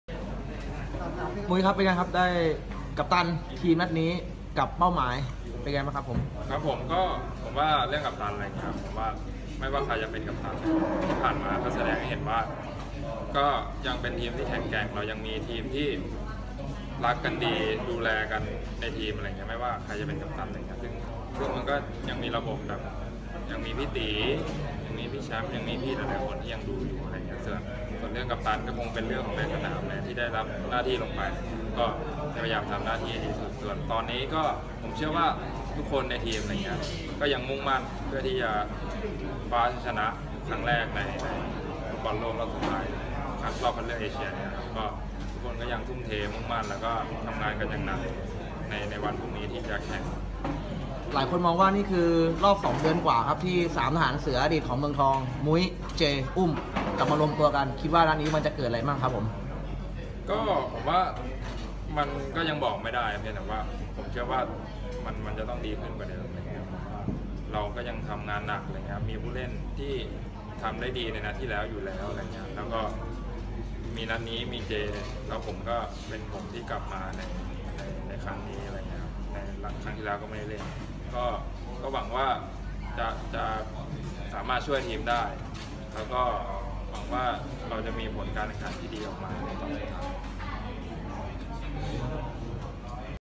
กัปตันมุ้ย พูดถึงการเจออิรัก พร้อมสภาพความพร้อมของทีม
กัปตันมุ้ย พูดถึงการเจออิรัก และการกลับมาของ 3 ทหารเสืออดีตกิเลนของเจ้าตัว, อุ้ม และ เจ